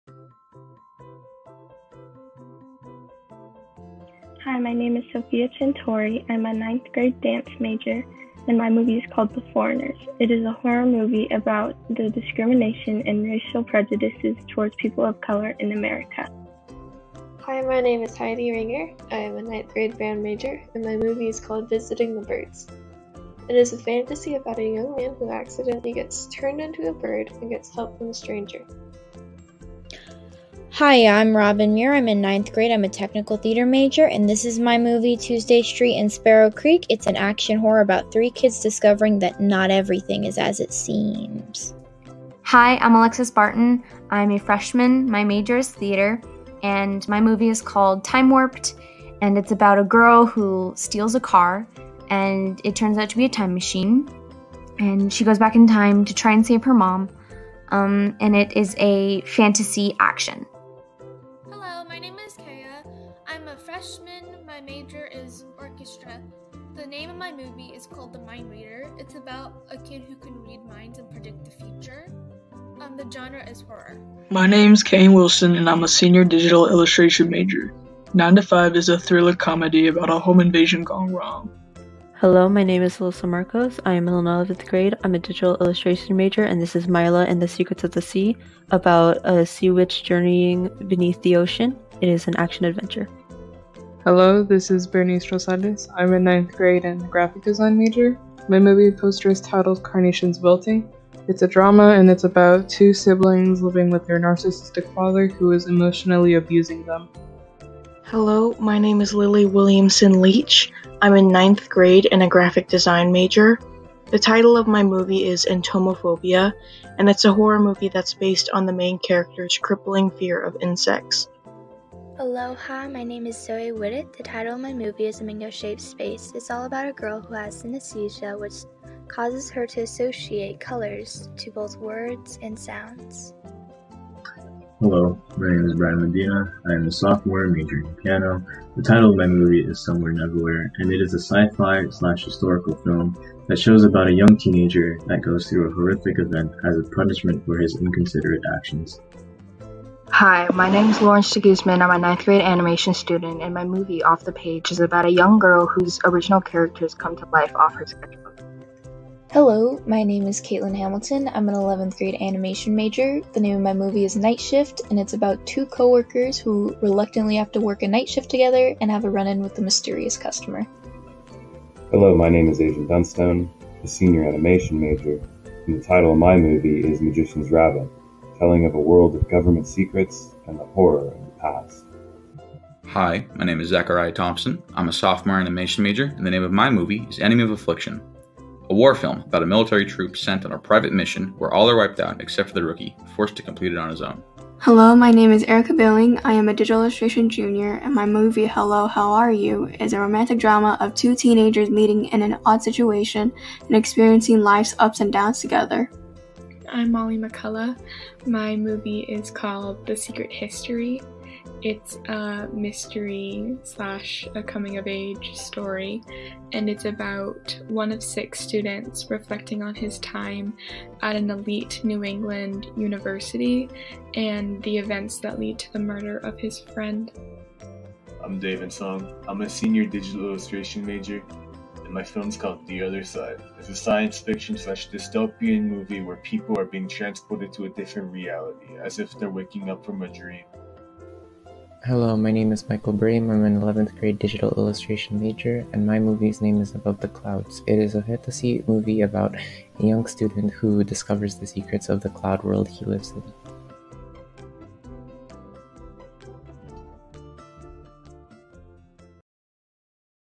LVA Graphic Design 1 - Movie Poster Designs
LVA Graphic Design 1 class presents their Movie Poster Designs - teacher Mrs. Ashley Stroud.